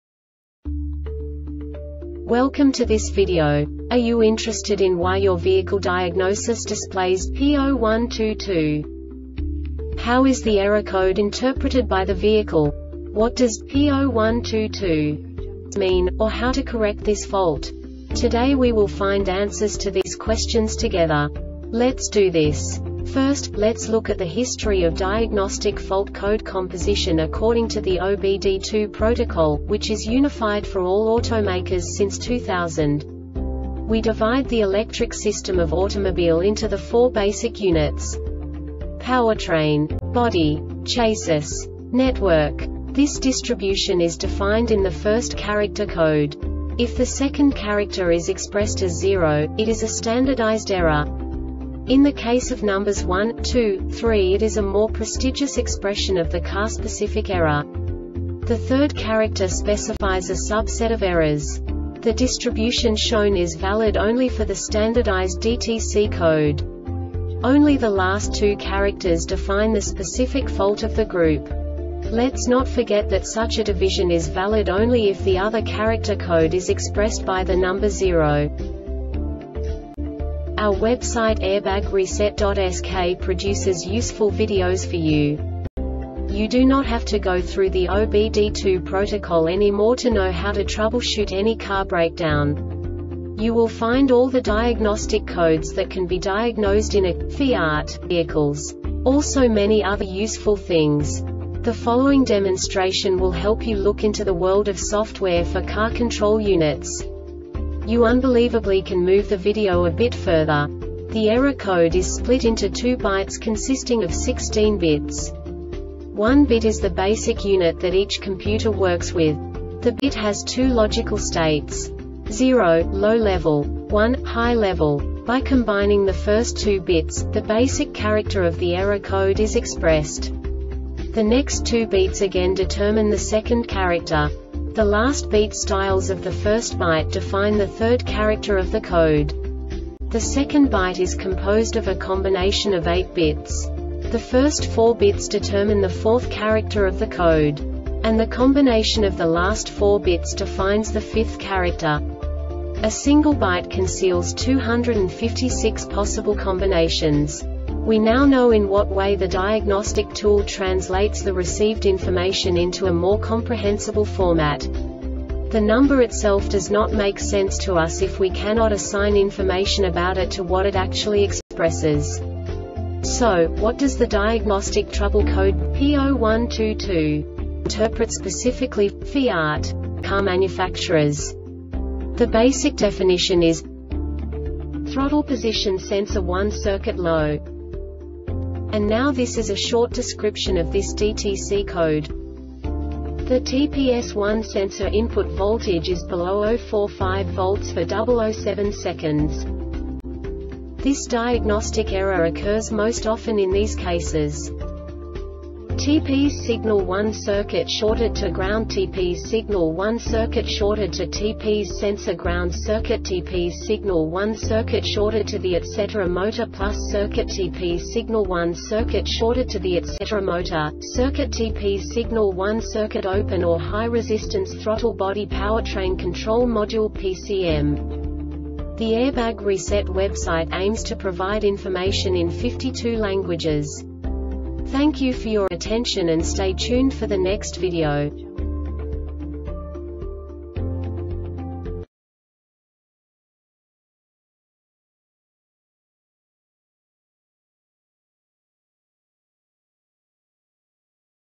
DTC Fiat P0122 Short Explanation
Contents:
0:21 Basic DTC analysis according to OBD2 protocol standard.
1:48 Insight into programming
2:58 A diagnostically understandable description of the Diagnostic Trouble Code
3:05 Basic error definition

Make:
FIAT
Code:
P0122
Definition:
Throttle Position Sensor 1 Circuit Low
Description:
The TPS 1 Sensor input voltage is below 0.45 volts for 0.07 seconds
Cause:
Tps Signal 1 Circuit Shorted To Ground
Tps Signal 1 Circuit Shorted To Tps Sensor Ground Circuit
Tps Signal 1 Circuit Shorted To The Etc Motor (+) Circuit
Tps Signal 1 Circuit Open Or High Resistance
Throttle Body